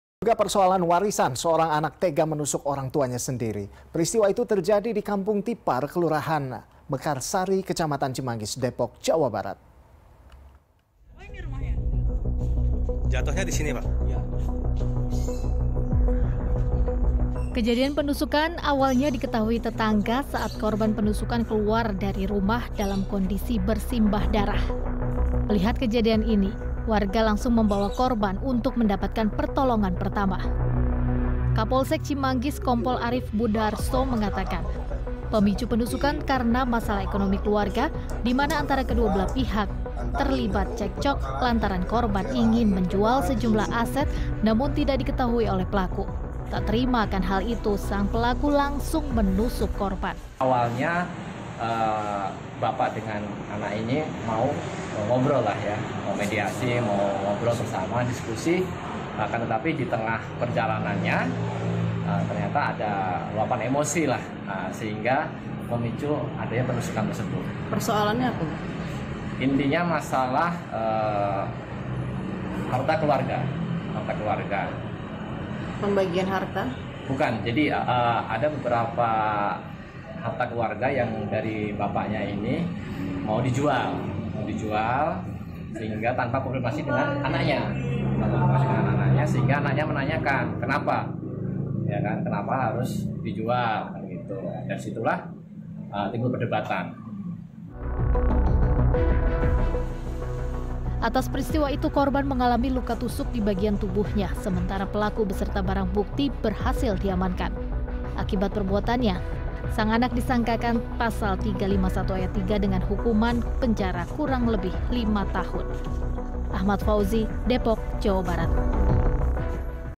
Anak di Depok Tega Tusuk Orang Tua Masalah Warisan
Diduga persoalan warisan, seorang anak tega menusuk orang tuanya. Peristiwa terjadi di Kampung Tipar, Kecamatan Cimanggis, Depok, Jawa Barat, Selasa 3 Oktober 2023 lalu. Kejadian penusukan awalnya diketahui tetangga, saat korban penusukan keluar rumah dalam kondisi bersimbah darah.

Tonton Beritasatu Pagi pukul 4.30 WIB.